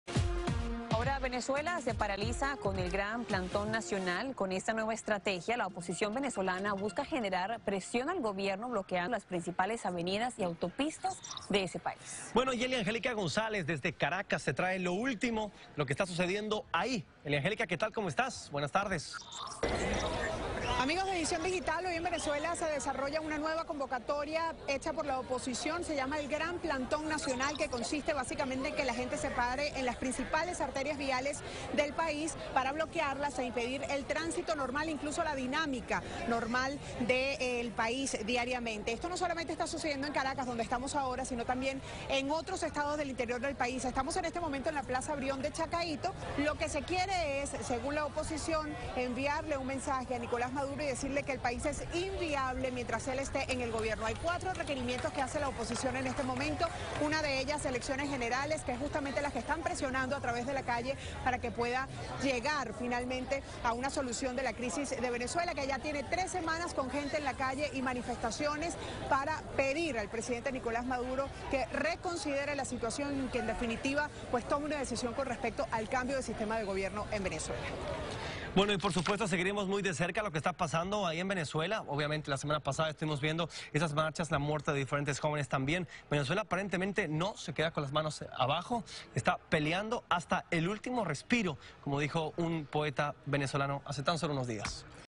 'Gran plantón nacional', nueva movilización de la oposición para presionar al gobierno venezolano
Este lunes los venezolanos vuelven a salir a protestar, esta vez bloqueando las principales calles del país. De esta manera se busca seguir presionando al gobierno. El objetivo es enviarle un mensaje a Nicolás Maduro, diciéndole que Venezuela es inviable mientras él esté en el poder.